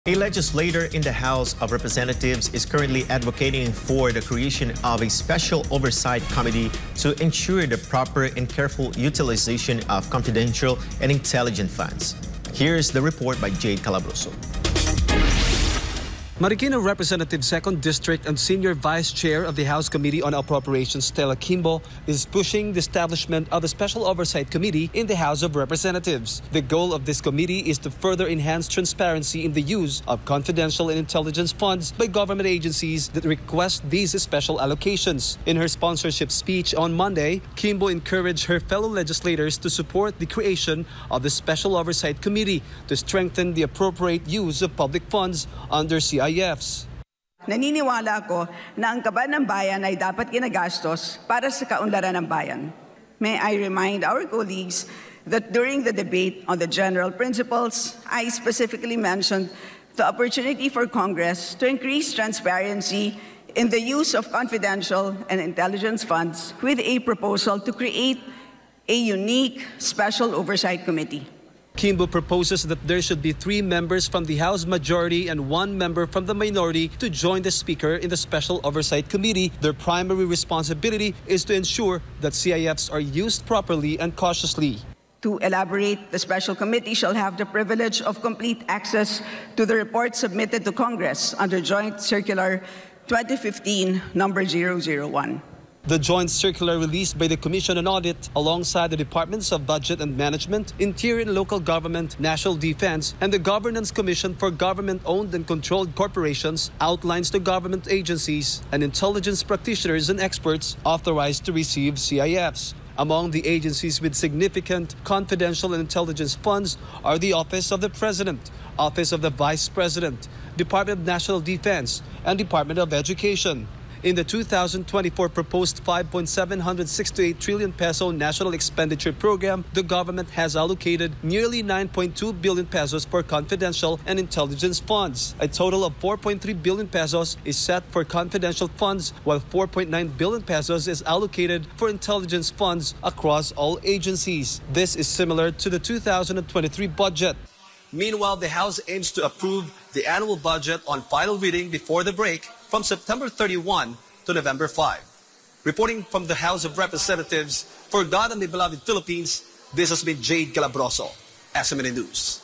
Lawmaker pushes for a 'special oversight committee' to ensure proper use of confidential funds
A legislator in the House of Representatives is currently advocating for the creation of a special oversight committee to ensure the proper and careful utilization of confidential and intelligence funds. | via Jade Calabroso